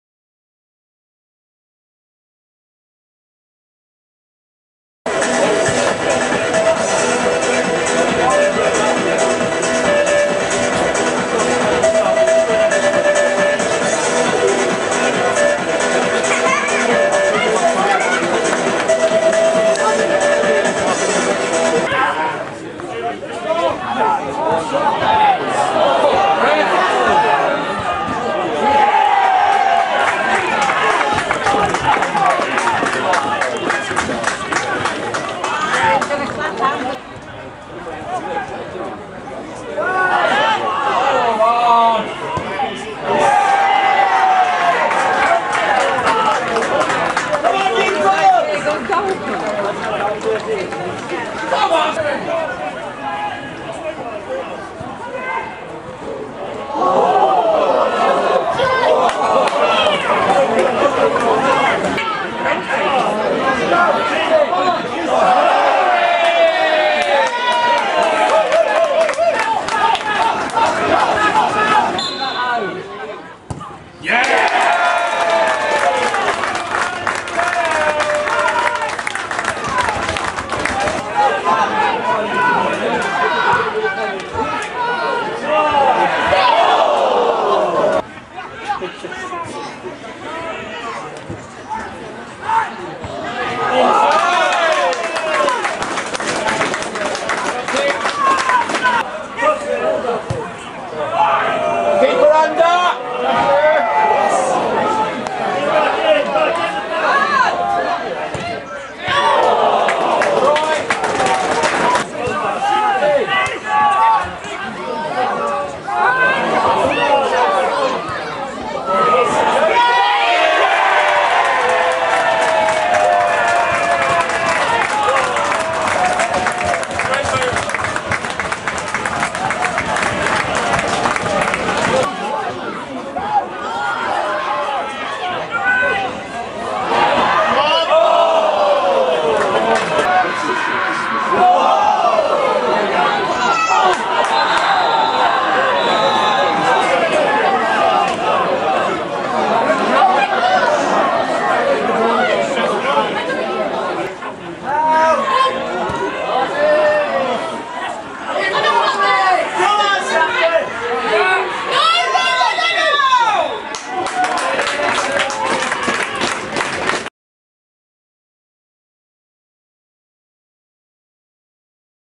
Stamford v WhitbyTown
Stamford's 2-2 draw with Whitby Town FC. Saturday January 23rd 2016.